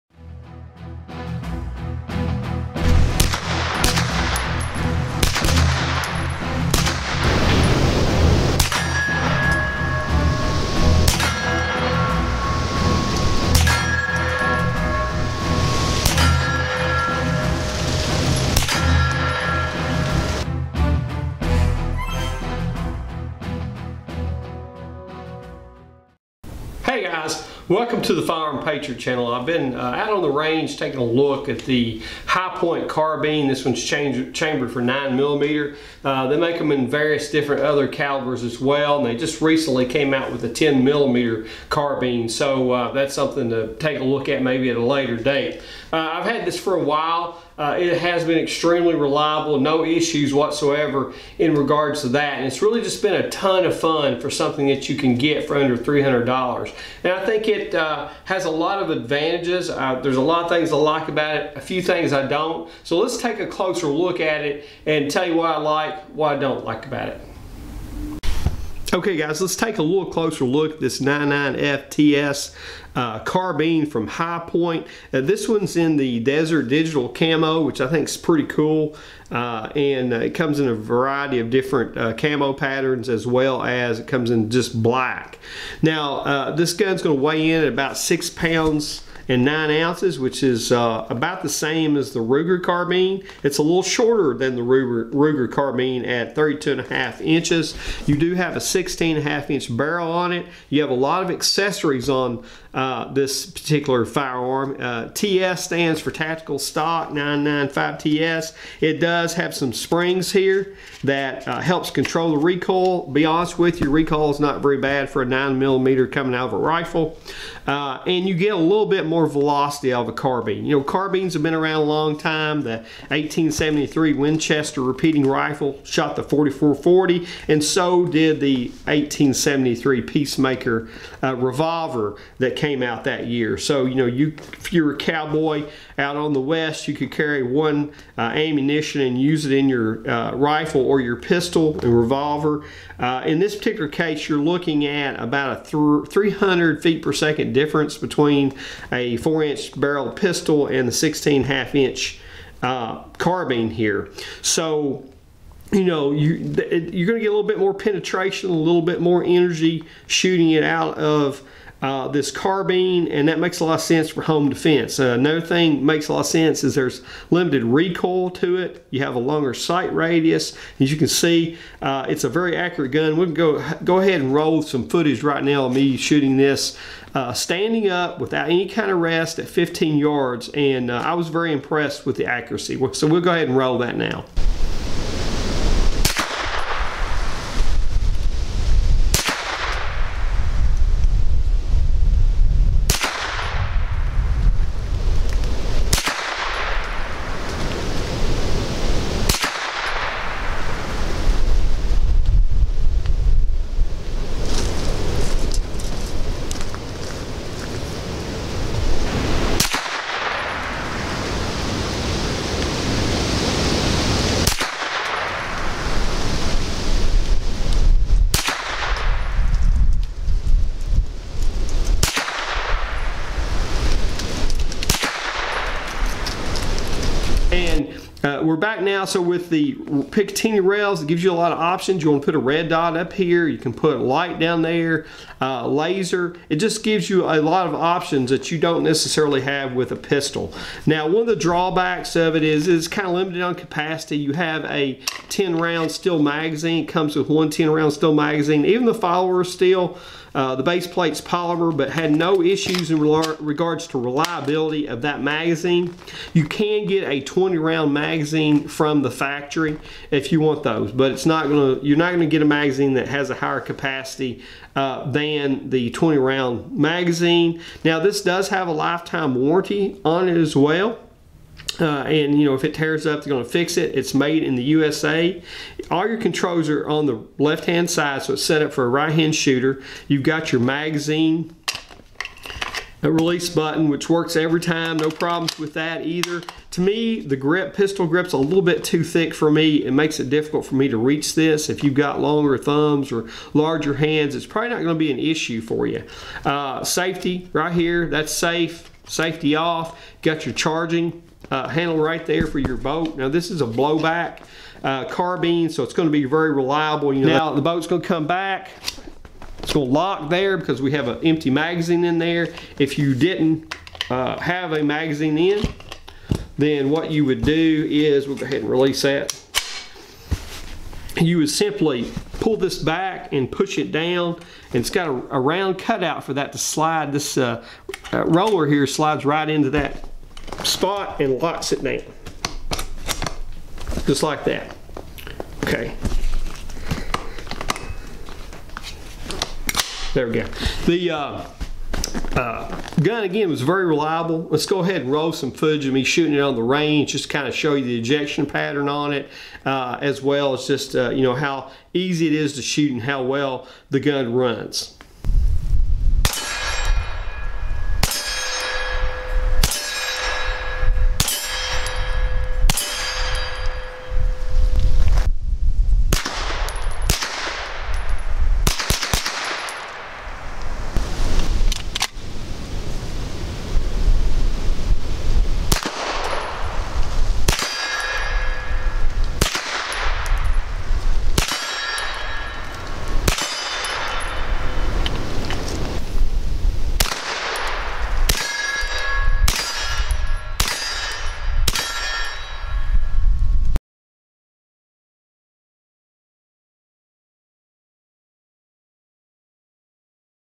HiPoint Carbine 9mm! It seems Reliable!
The HiPoint Carbine chambered for 9mm seems to be a very reliable platform with its blowback system.  There are a few things I would change about it but I know of no other carbines that can be purchased for this price.  It is also fun to shoot!